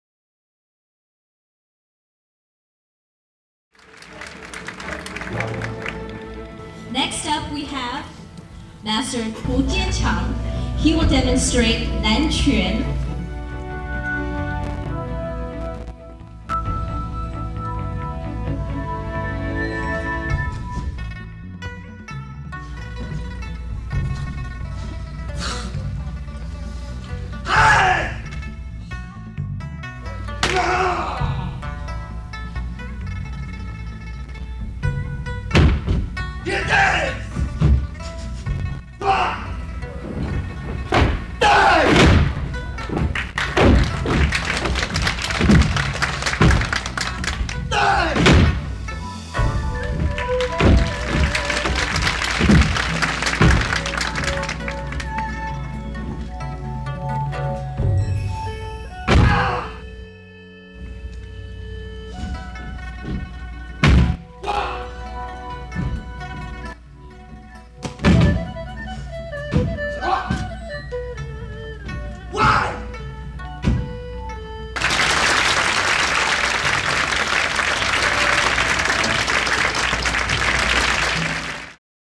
Master Hu Jianqiang's Nanquan form
Master Hu Jianqiang demonstrates his Nanquan form at the Kung Fu Tai Chi 10th Anniversary Gala Event.